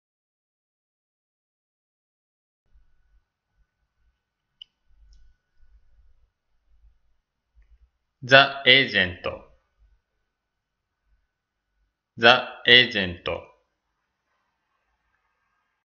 Jerry Maguire - Movie Title in Japanese
Jerry Maguire (1996) is  The Agent (ザ・エージェント) in Japanese Title.

Fifth option (Different title in English) applies here.

5 Ways to Translate Titles in Japan

#1. Use the original title
#2. Literal translation of original title
#3. #1 or #2 with subtitle
#4. Different title in Japanese
#5. Different title in English

Pronunciation in videos is actual pronunciation in Japan. (In case the title is in Japanese, I read as is.)